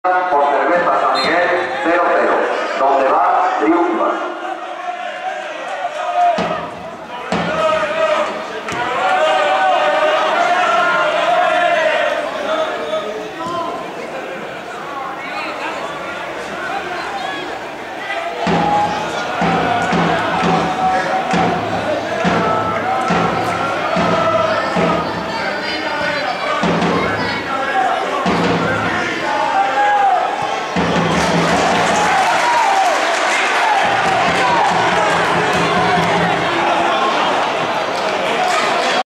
Mal estado del césped (Extremadura UD 1-1 Ciudad Plasencia) J14 '12-'13
Por enésima vez durante el partido, la jugada de ataque del Extremadura queda en nada por el mal estado del terreno de juego. Esta vez le bota mal al capitán Ruiz. Partido correspondiente a la 14ª Jornada de Liga en el grupo XIV de 3ª División. Disputado el domingo, 25 de noviembre de 2012 en el 'Francisco de la Hera' de Almendralejo (Badajoz).